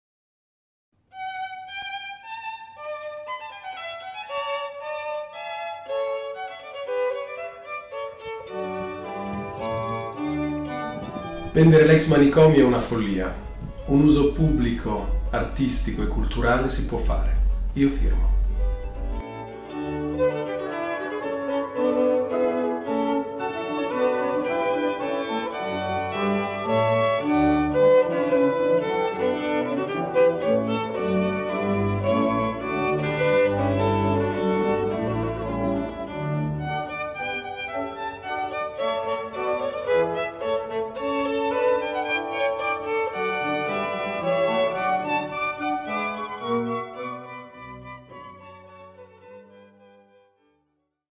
Stefano Accorsi per "Si può fare", Io Firmo!
l'Attore Stefano Accorsi sostiene la campagna "Si può fare!" io firmo per il S.Maria della Pietà Bene Comune